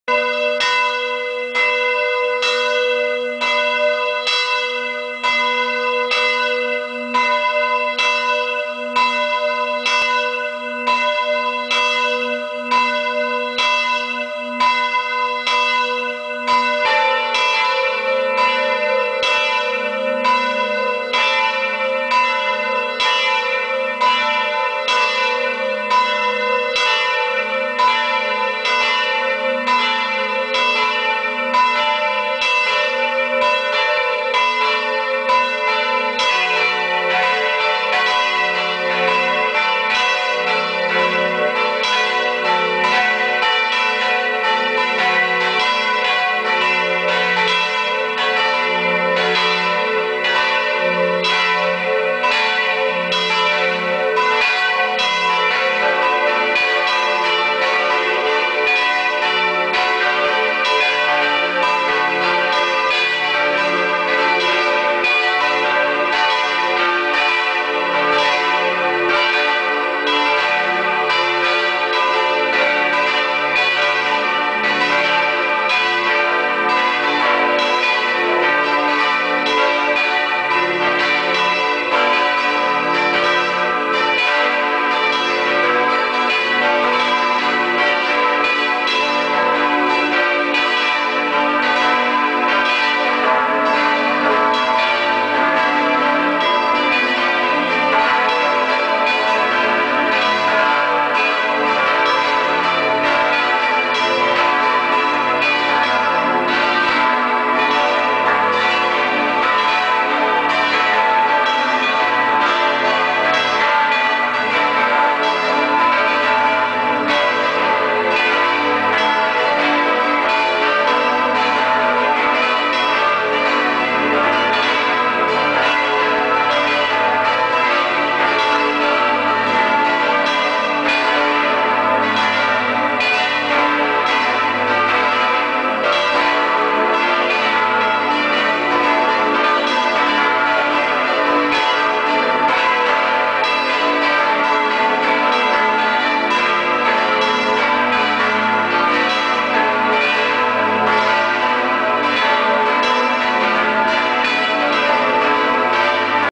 CH - Sumvitg/Somvix (GR) Baselgia/Pfarrkirche
Baselgia Parochiala Gion Battesta e Mudest.

Geläute der Pfarrkirche St. Johannes Baptista und hl. Modestus in Somvix, Kanton Graubünden.

6 Glocken von Rüetschi/Aarau 1930/33
A° H° d' fis' a' h'
Grosse Glocke von 1933, restliche von 1930